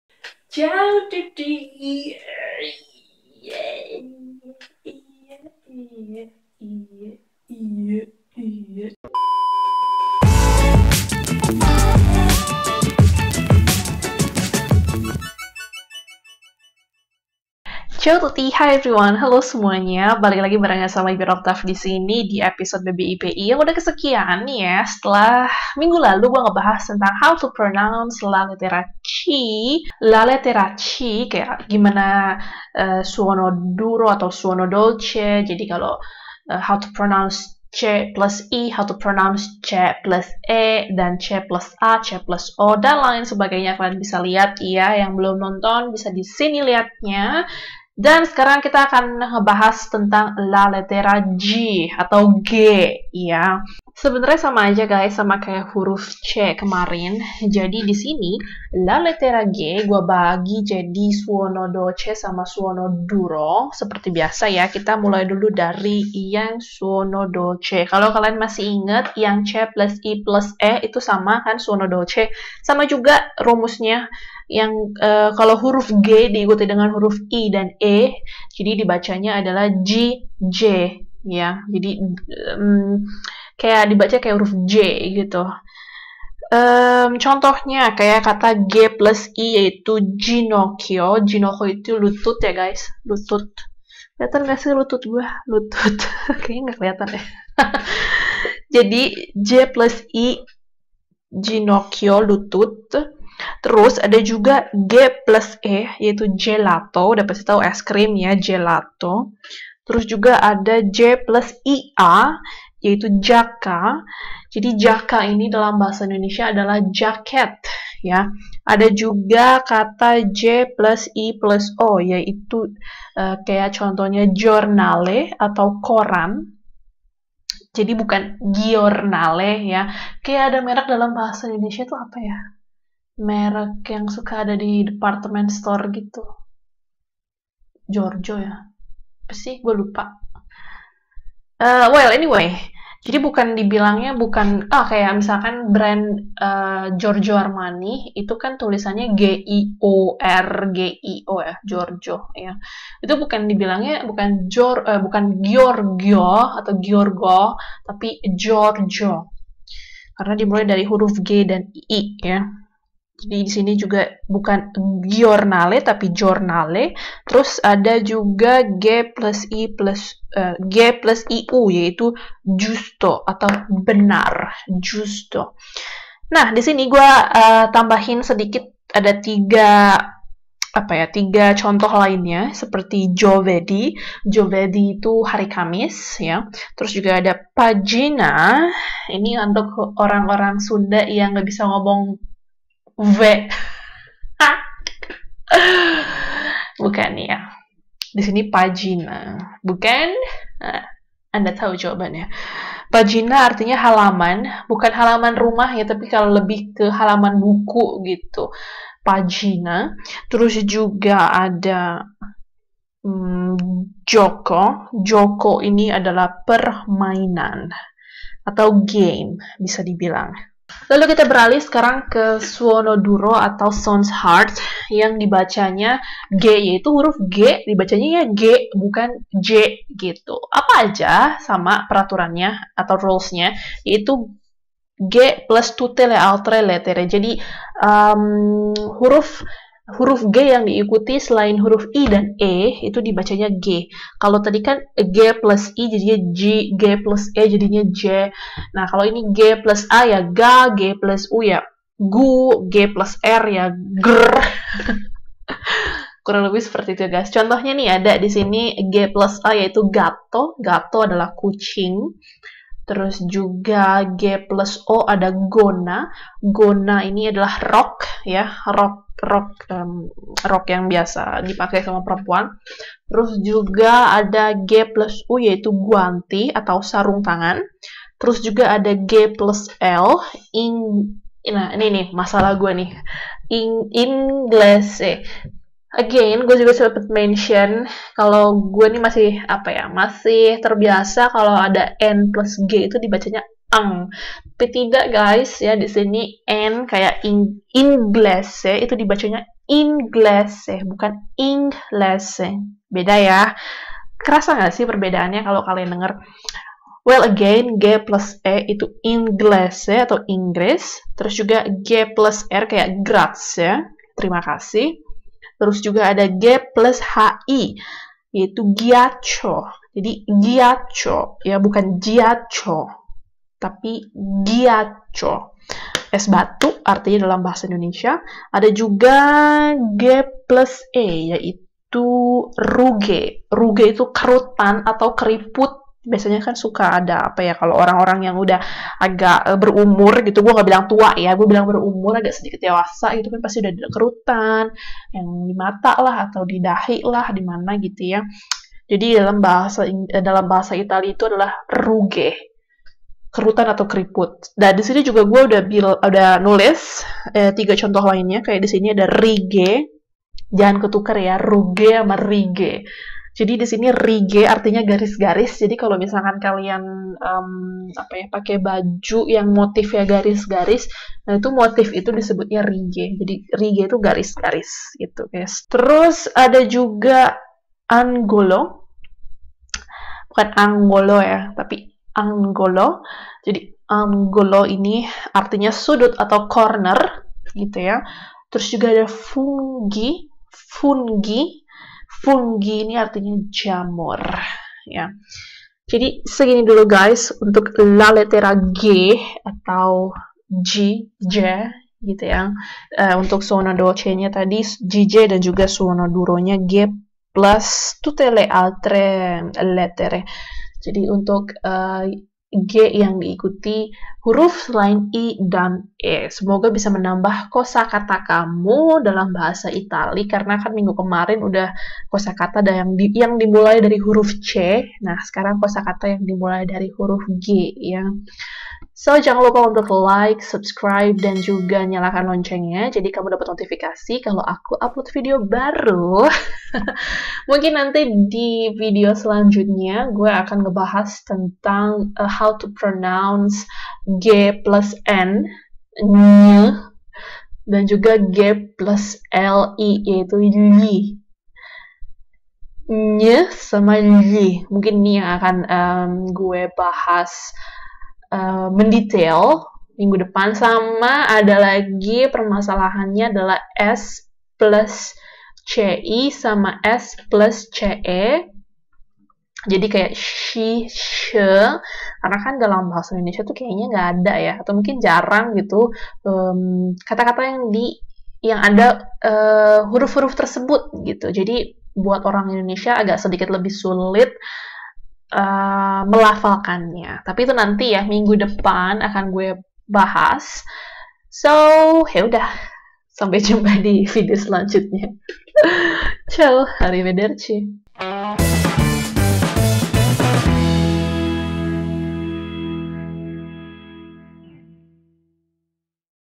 BELAJAR BAHASA ITALY BARENG IBETH #14 | HOW TO PRONOUNCE LA LETTERA "G"
Ciao a tutti. Hi everyone. Halo semuanya!
Episode kali ini simple banget, gimana cara menyebutkan la lettera "G". Semoga gak bingung lagi untuk mengucapkan kata-kata yang dimulai dari huruf "G". Btw untuk yang mau bergabung di group Telegram, silahkan klik